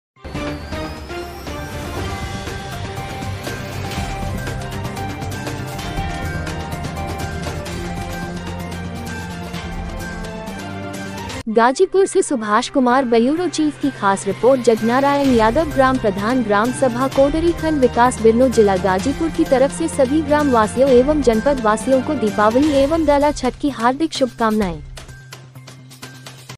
गाजीपुर-जगनारायण यादव ग्राम प्रधान ग्राम सभा कोडरी खंड विकास बिरनो जिला गाजीपुर की तरफ से सभी ग्राम
गाजीपुर-जगनारायण यादव ग्राम प्रधान ग्राम सभा कोडरी खंड विकास बिरनो जिला गाजीपुर की तरफ से सभी ग्राम वासियों एवं जनपद वासियों को दीपावली एवं डाला छठ की हार्दिक शुभकामनाएं।